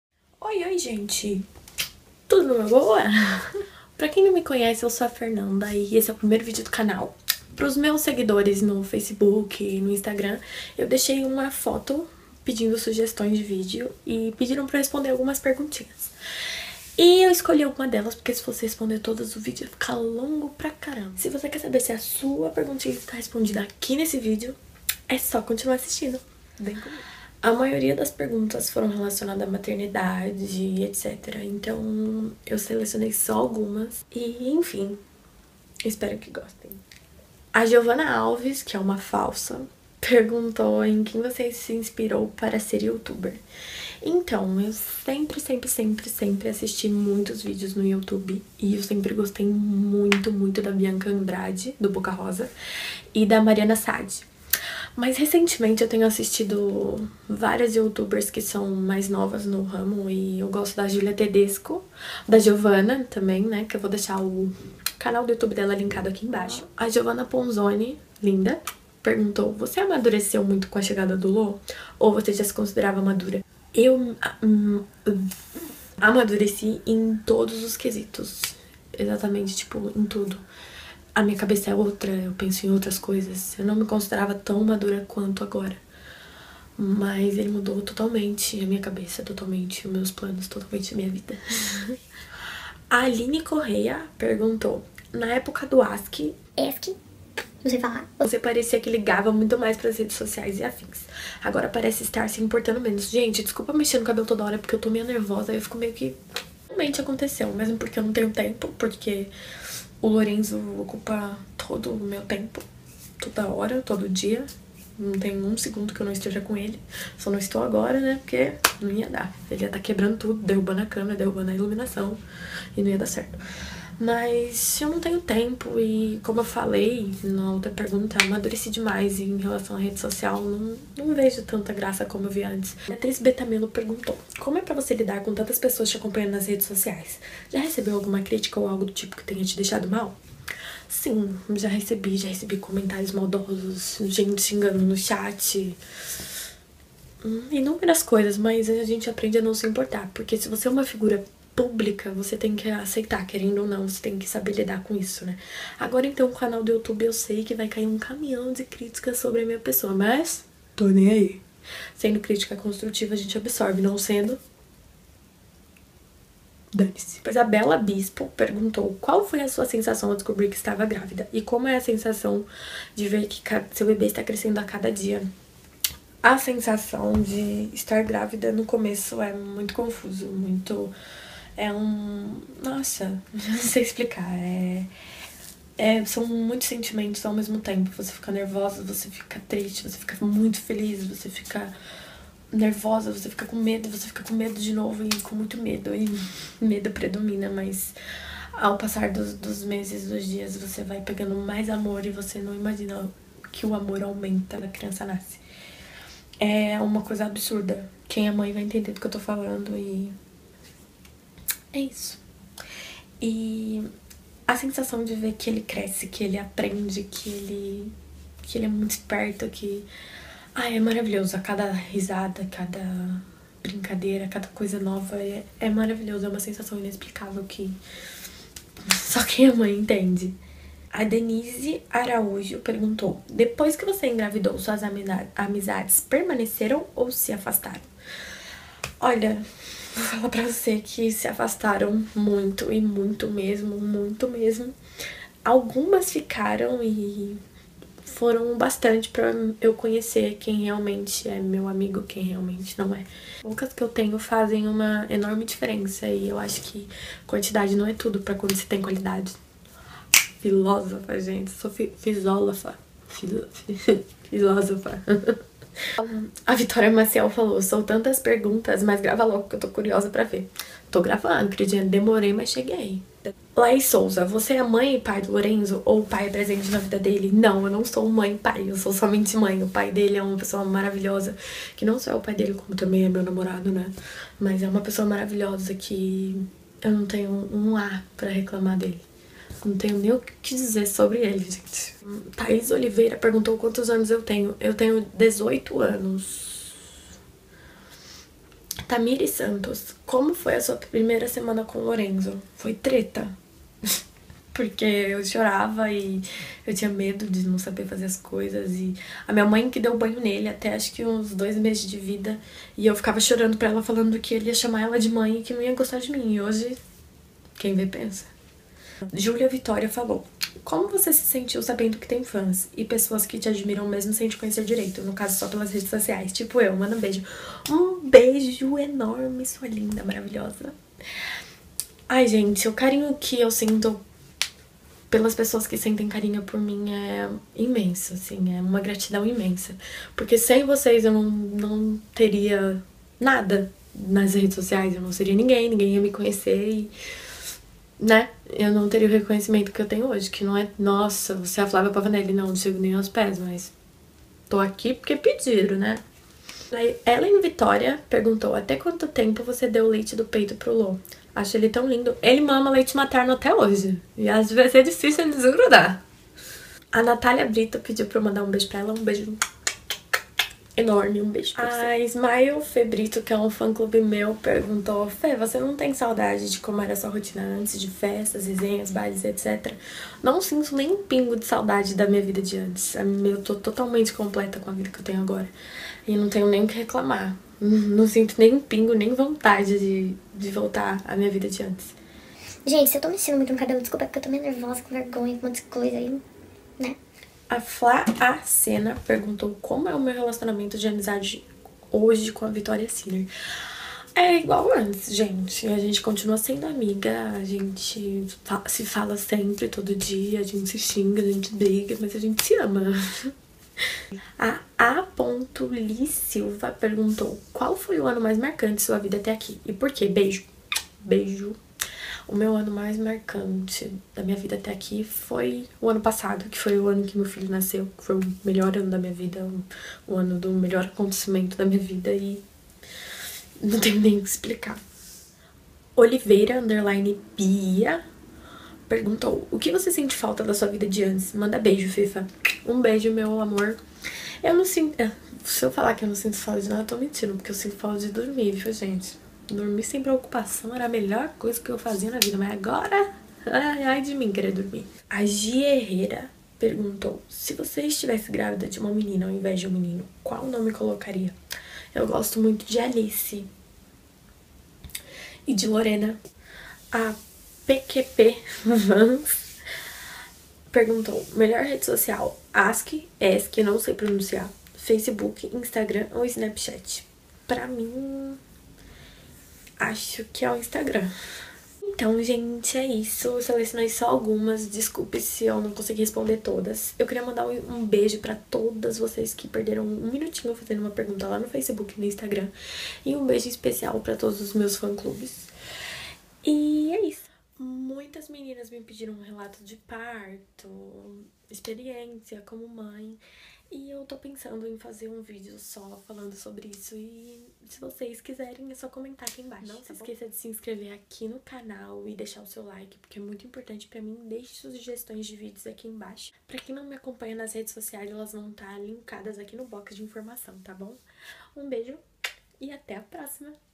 FernandaResponde | Fernanda Brito
Desculpem o nervosismo e alguns erros. Infelizmente não consegui responder todas as perguntas pois eram muitas, espero que entendam!
Deixem sugestões aqui em baixo para os próximos vídeos =)